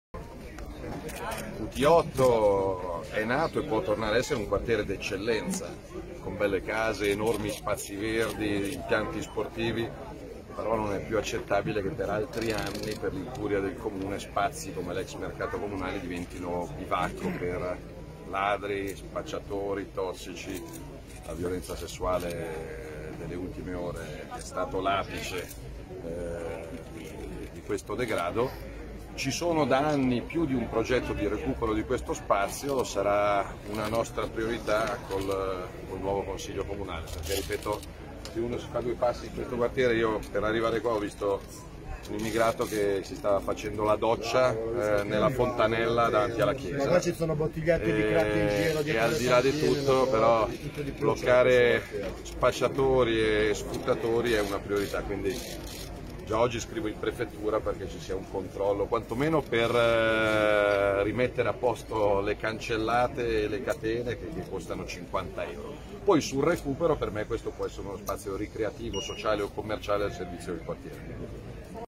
Salvini: "Basta degrado al QT8 di Milano" (20.07.20)
- Salvini: "Basta degrado al QT8 di Milano. Deve tornare ad essere un centro al servizio del quartiere". (20.07.20)